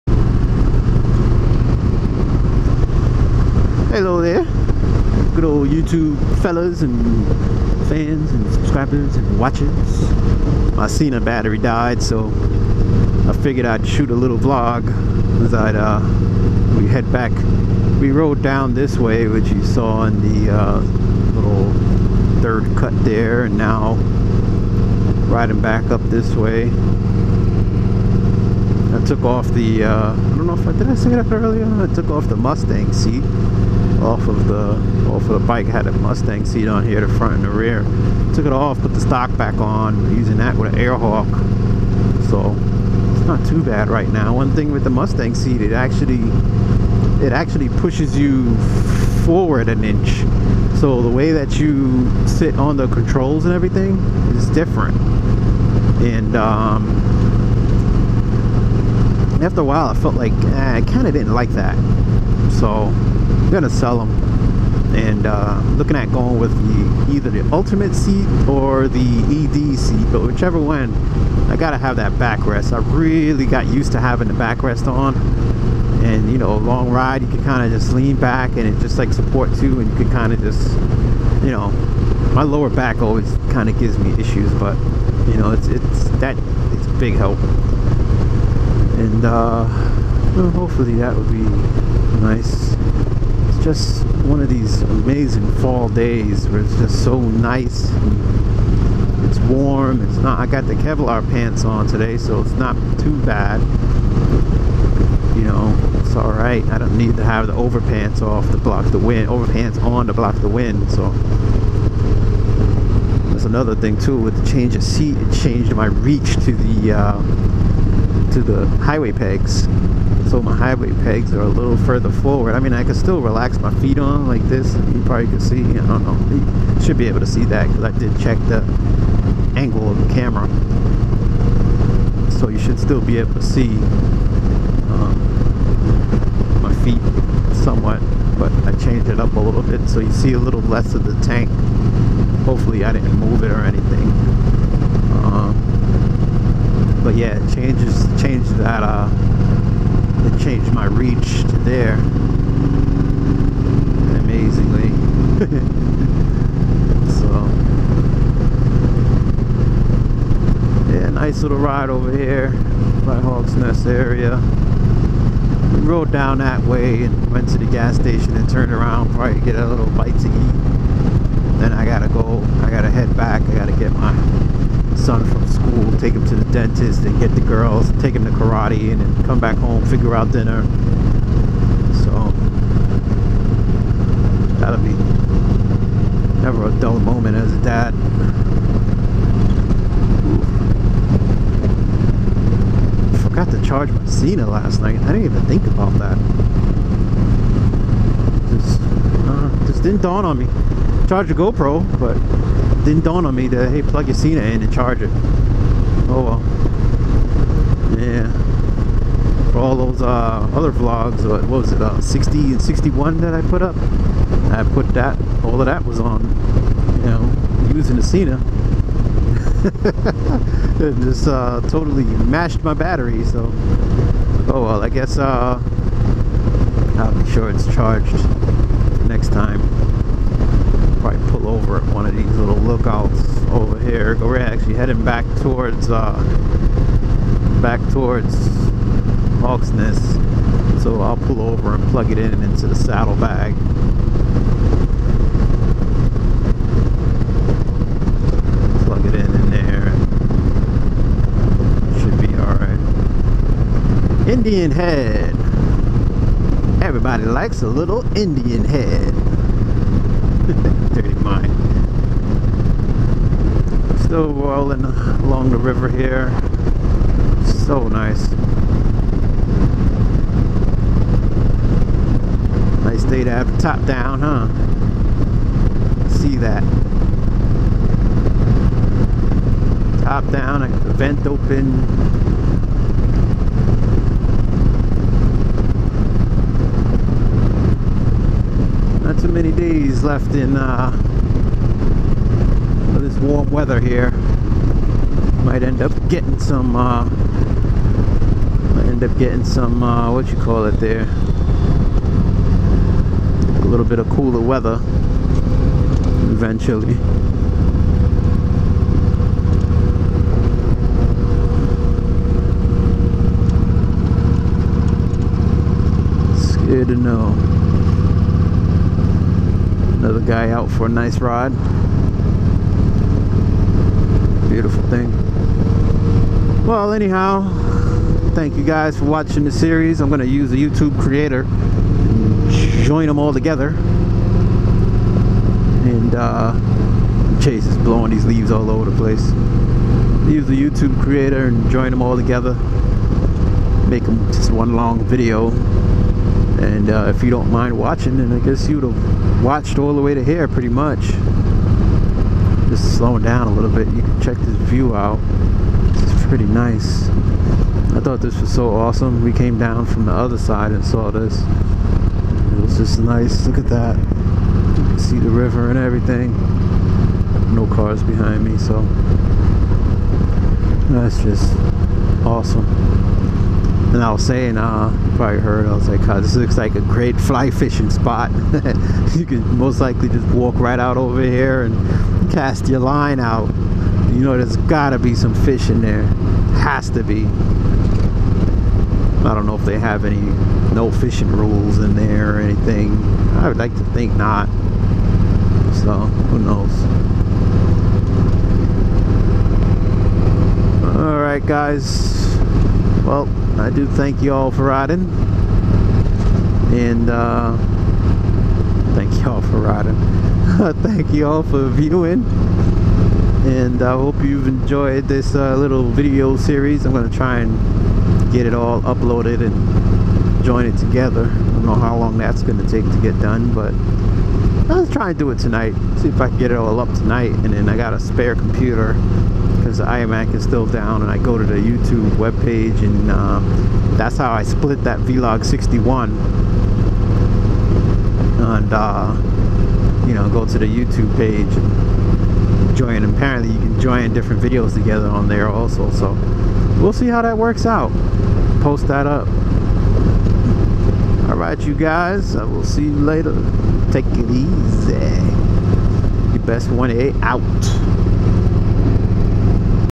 Vlog #65 a little vlogging + lovely scenery + no more Mustang seats + read descrip section 4 of 4.
My sena went dead on me.. dunno why lol. I guess I forgot to charge it. Not sure how I forgot that. Making a seat change from the mustang seats over to the ultimate seat . I nailed a rear seat on ebay .. and will buy the ultimate front seat with backrest next season I guess. right now I'm on the stock seat with a airhawk air pad which has been working wonders ..
mac is still down, but thanks to bikerboyCb900c for the wonderful help even though i was slow as molasses in responding on yt messenger. A big thank you ..